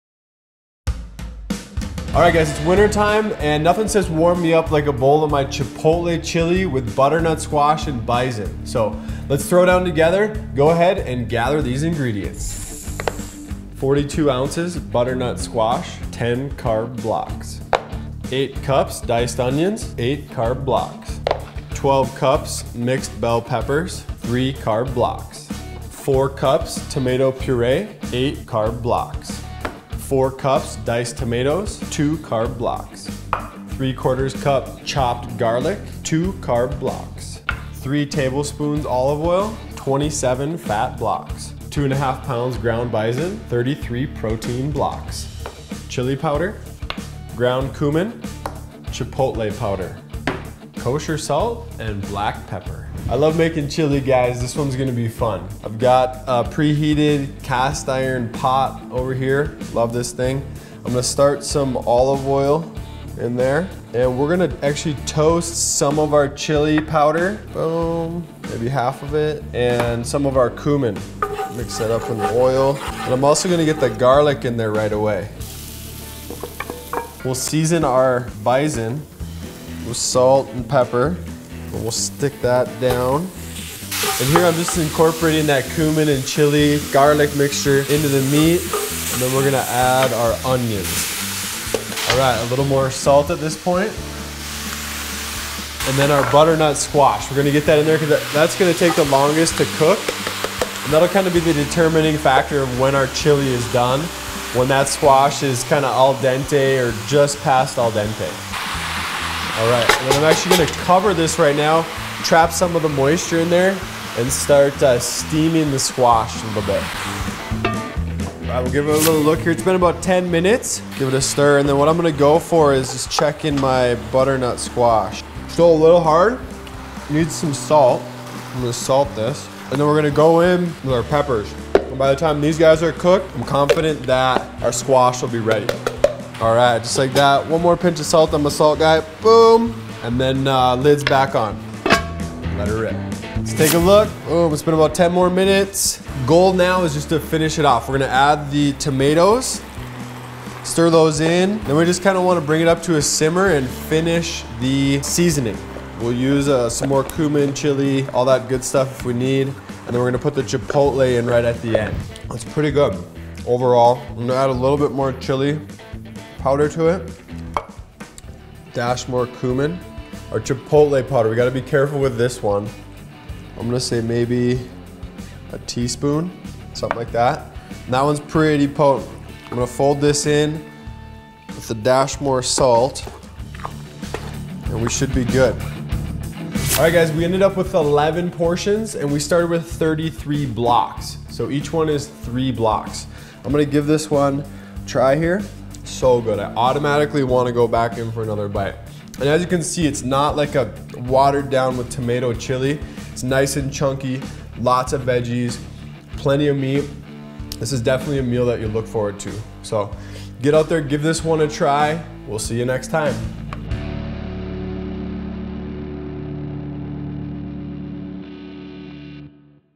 Nick’s Zone: Chipotle Bison Chili With Butternut Squash
In these chilly winter months, nothing hits the spot like a bowl of hearty chili. Nick Massie of PaleoNick.com brings us this simple recipe to warm your soul—and your belly.

“Nothing says ‘warm me up’ like a bowl of my chipotle chili with butternut squash and bison,” Massie says.

You’ll only need one pot and these fresh ingredients: butternut squash, onions, bell peppers, tomato puree, diced tomatoes, garlic, olive oil, bison, chili powder, cumin, chipotle powder, salt and pepper.

Start by toasting the chili powder and cumin in olive oil. Toss in the garlic and seasoned meat and begin incorporating the onions, butternut squash and peppers as directed. Allow the ingredients to steam with the lid on for a bit and you’ll be ready to add the tomato product and, finally, the chipotle.

After portioning out 11 three-block meals, Massie tests the dish to ensure it’s perfect.

“So good,” he says. “I automatically want to go back in for another bite. … This is definitely a meal that you’ll look forward to.”

To download the recipe for chipotle bison chili with butternut squash,

Video by Nick Massie and Jesse Kahle.